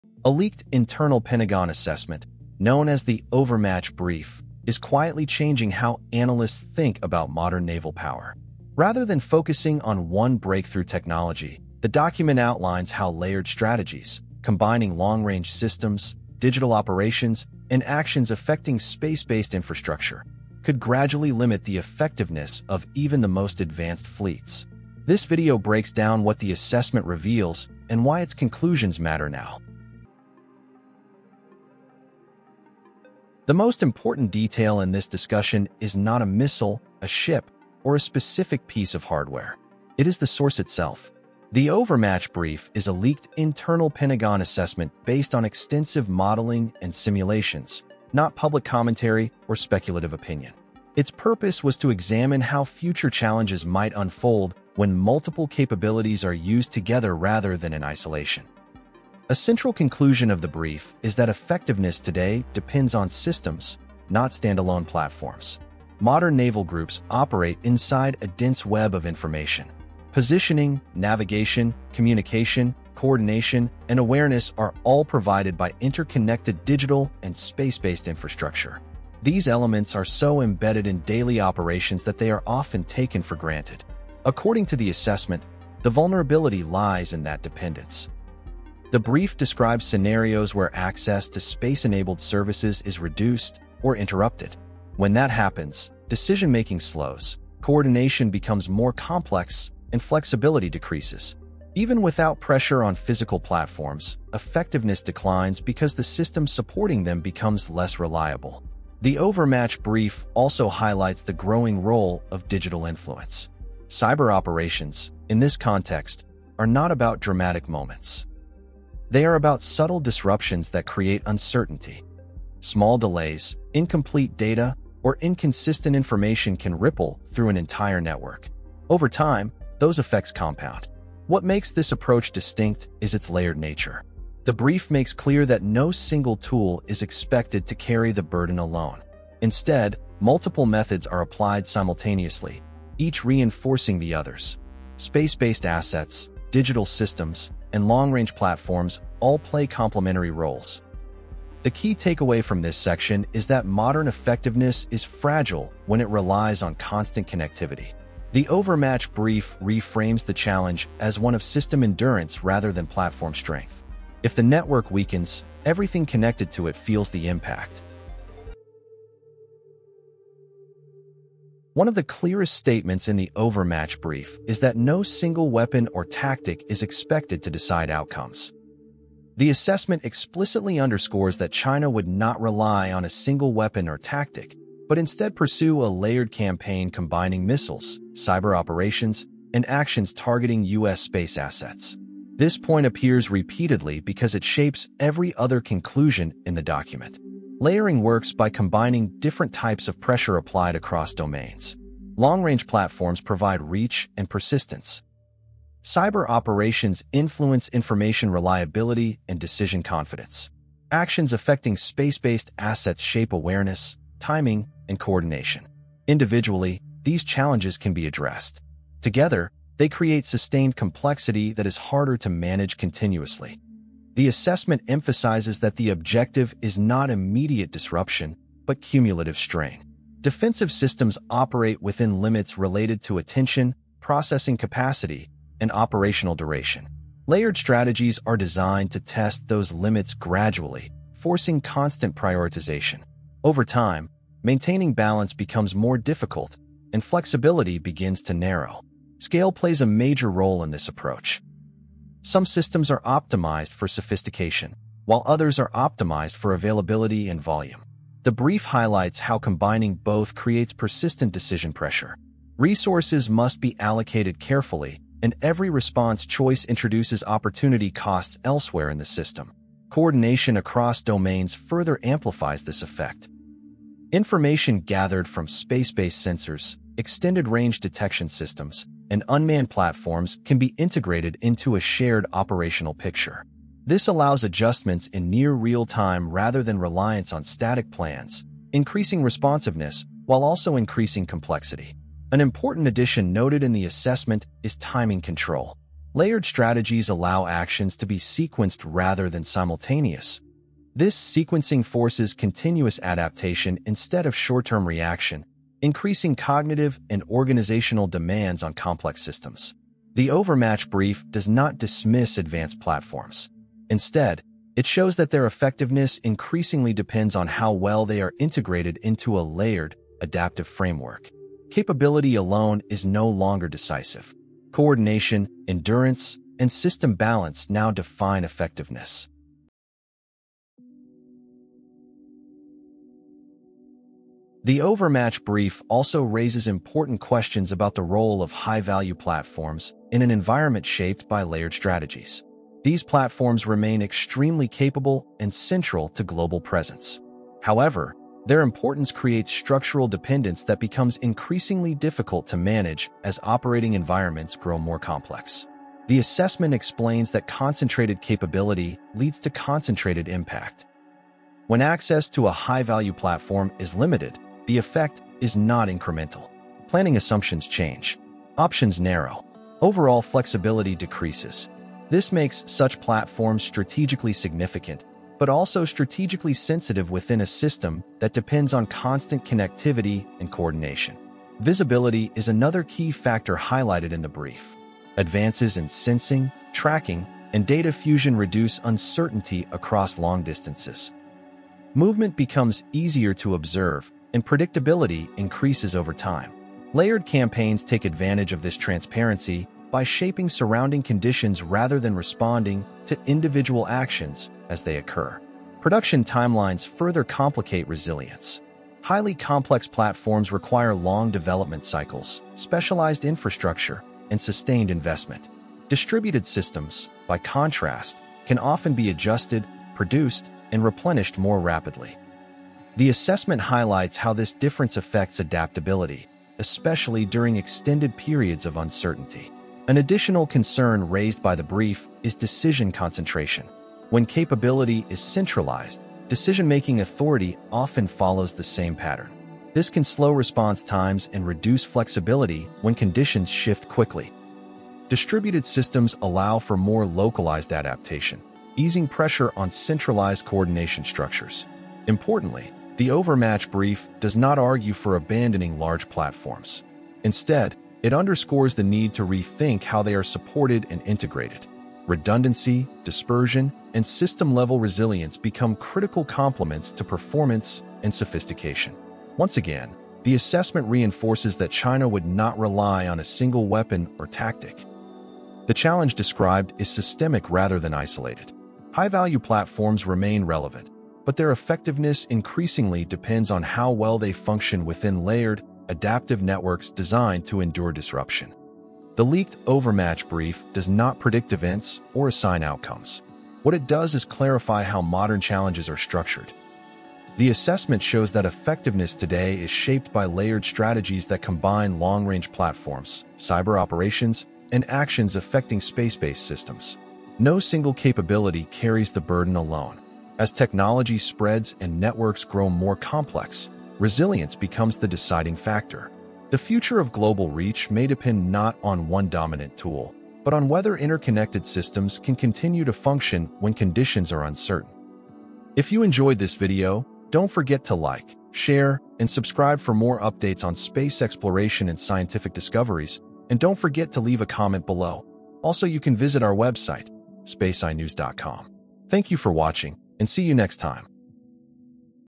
Pentagon Study Reveals How China Could Challenge U.S. Aircraft Carriers!
A leaked internal Pentagon assessment known as the Overmatch Brief is reshaping how experts think about modern naval power. Based on advanced simulations, the study suggests that China would not rely on a single weapon or tactic. Instead, it highlights a layered approach combining missiles, cyber operations, and actions affecting U.S. space assets to place sustained pressure on complex systems.
Rather than focusing on dramatic moments, the assessment shows how coordination, scale, and system endurance could gradually limit effectiveness over time. Space-based infrastructure, digital networks, and long-range platforms all play interconnected roles, meaning resilience now matters as much as raw capability.
In this video, we break down what the Overmatch Brief actually says, why aircraft carriers are central to the discussion, and how this assessment changes long-standing assumptions about global reach. This is not speculation — it’s how modern strategy is being quietly re-evaluated.
 Reference :
🚀 Space tech. Satellites. Innovation.